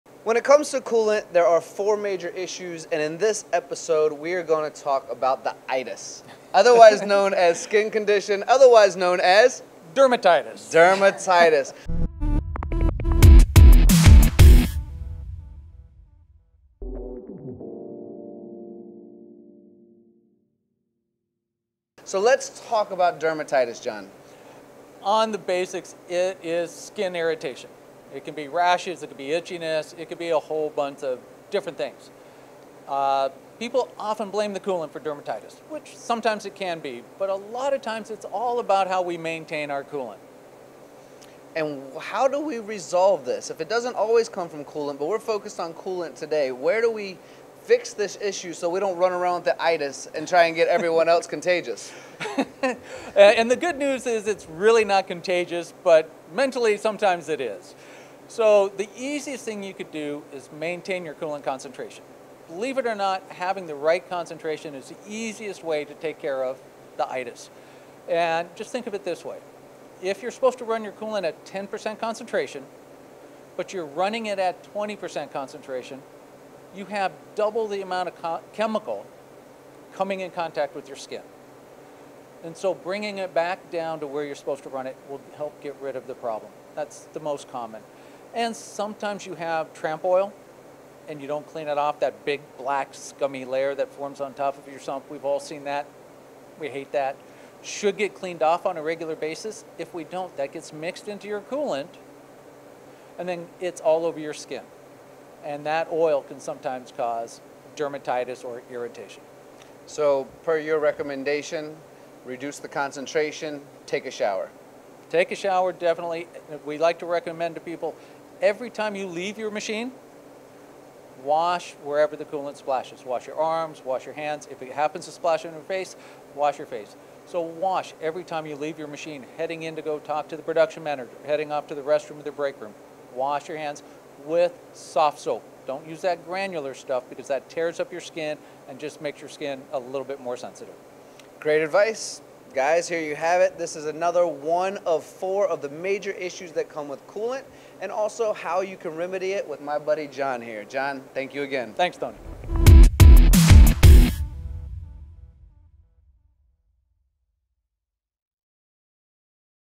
Dealing with Dermatitis - Featuring John Treese, and Tony Gunn of MTDCNC
John Treese, Global Training Director, talks with Tony Gunn from MTDCNC about what dermatitis is and how people often blame the coolant for dermatitis when the problem is actually coolant management. The easiest thing to prevent dermatitis is to have the right concentration. For example, if you're supposed to run your coolant at 10% concentration, but you're running it at 20% concentration, you have double the amount of chemical coming into contact with your skin so bringing it back down will help prevent dermatitis.

Be sure to follow Master Fluid Solutions on social media!